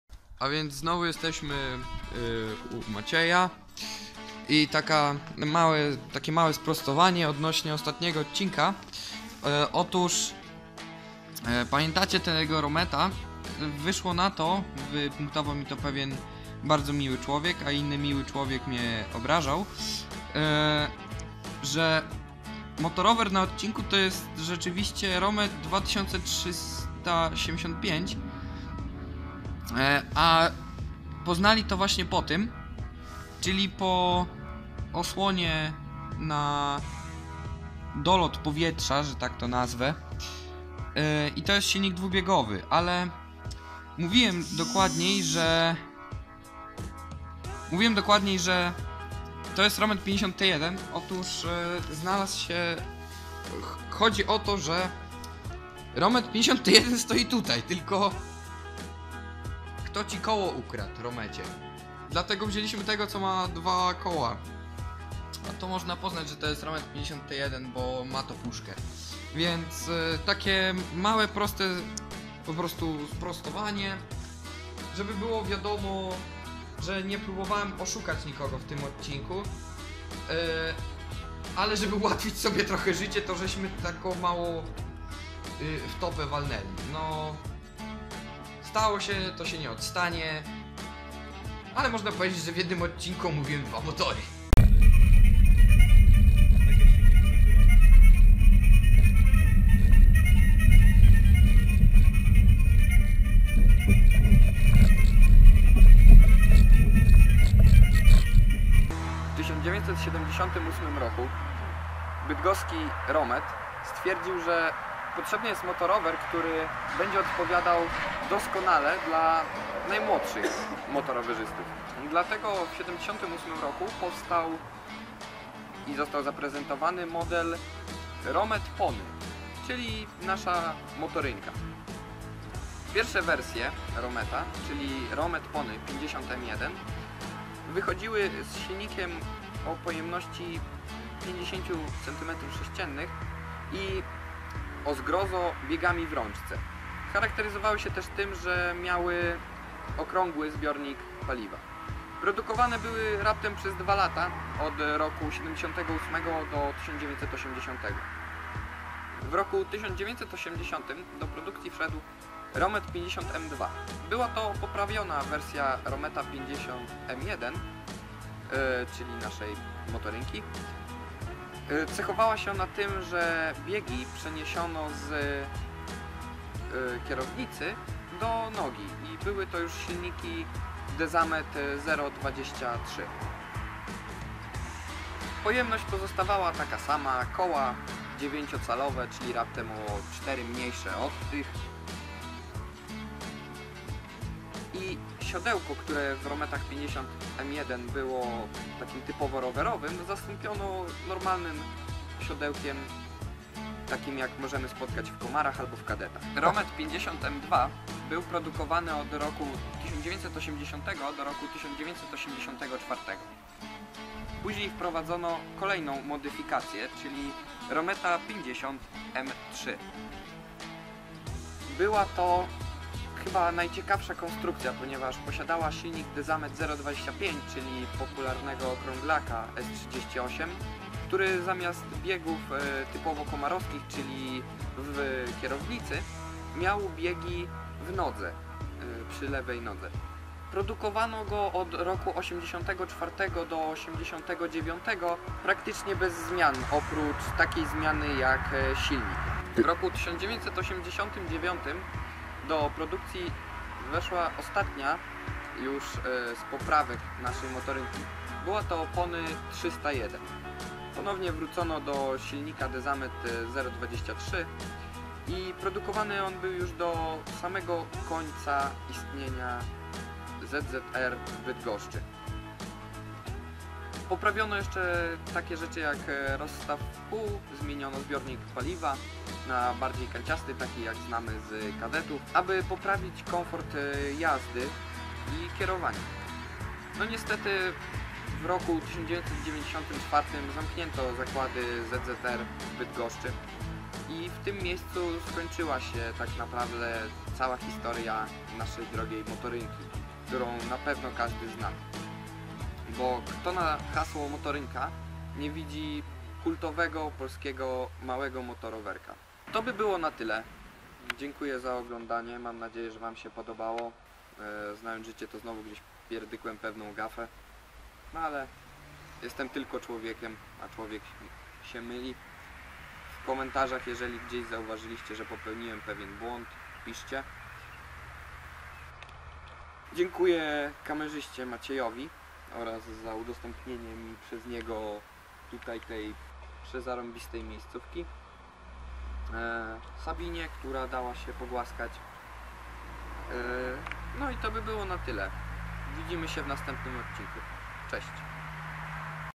Technika Minionych Lat - Odc.10 Romet Pony
Pozdrawiam Kubę Koryckiego który udostępnił motorynkę :)

Ojojoj długo się zeszło ale wracam, teraz tylko trochę się ogarnąć i daj panie Boże za 2 tygodnie kolejny odcinek, tym razem z samochodem ;)